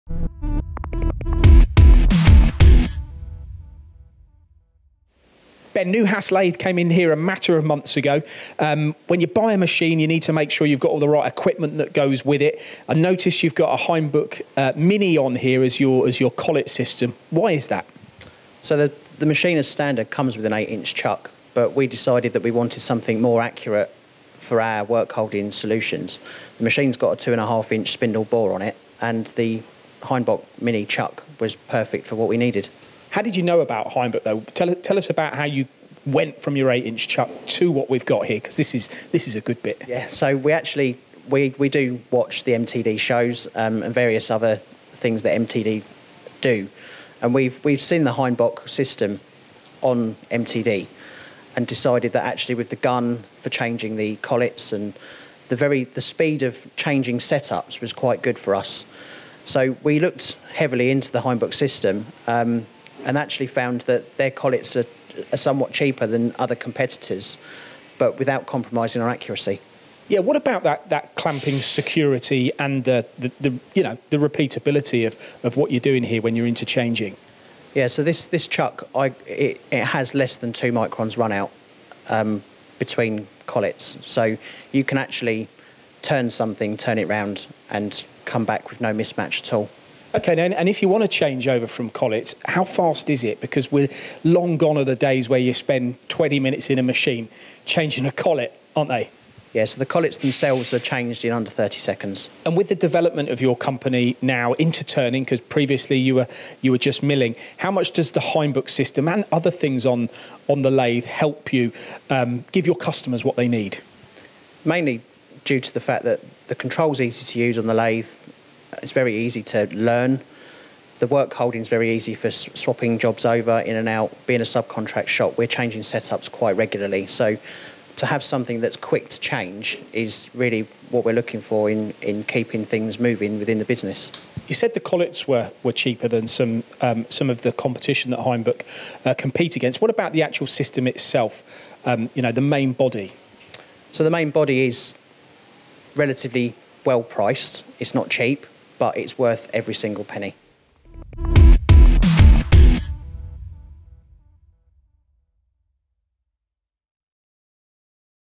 Prospeed Precision use the Hainbuch Mini as their collet system
Paul Jones from MTDCNC speaks to Ben Wesley from Prospeed Precision to ask about why they prefer using a Hainbuch Mini as their collet system and how it’s a good fit for the business. Ben shares insight into the pricing and performance of the Hainbuch. Prospeed Precision have looked heavily into the Hainbuch and watched the system on MTD. The Hainbuch’s control is easy to use, and the lay is very easy to learn. Being a subcontractor, Prospeed Precision change setups quite regularly so this product turned out to be the right equipment, giving them great speed of changing setups. Collets can be changed under 30 seconds. Although the collets are quite cheap compared to other competitors, the main body is well priced but worth every penny. Watch the video to know what makes this product the right fit for machining.